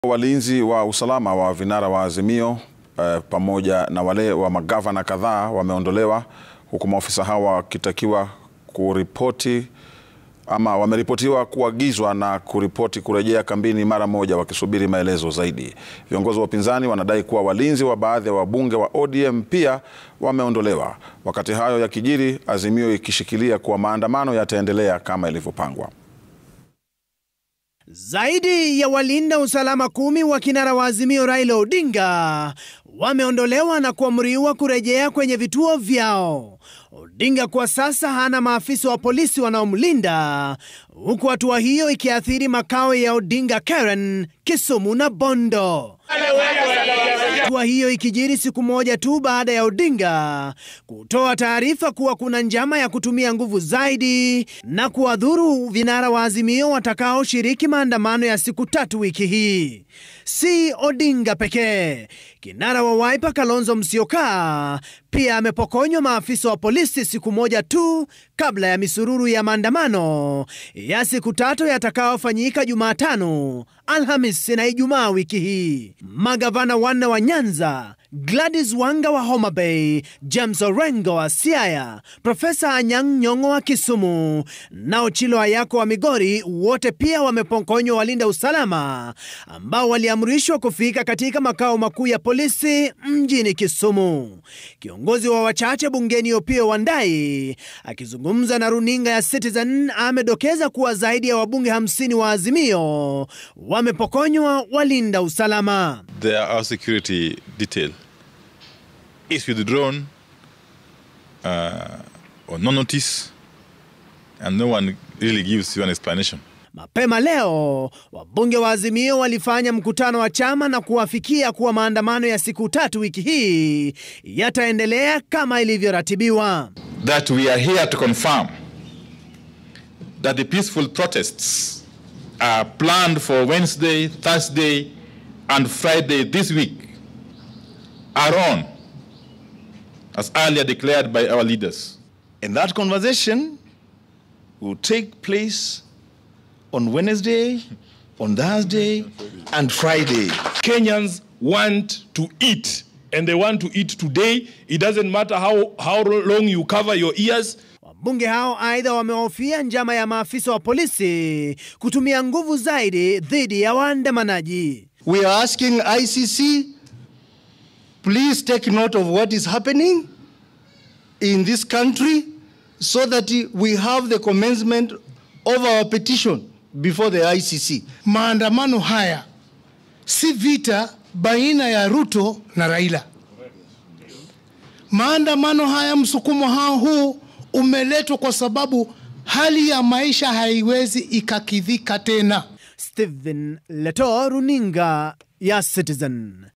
Walinzi wa vinara wa Azimio waondolewa
Walinzi wa usalama wa vinara wa Azimio Raila Odinga na Kalonzo Musyoka, pamoja na wale wa magavana kadhaa wameondolewa. Maafisa hao wa usalama wameripotiwa kuagizwa na serikali kurejea kambini mara moja wakisubiri maelekezo zaidi. Aidha, viongozi wa upinzani wanadai kuwa walinzi wa baadhi ya wabunge wa ODM pia wameondolewa. Stephen Letoo anaarifu huku Azimio ikishikilia kuwa maandamano yataendelea kama ilivyopangwa.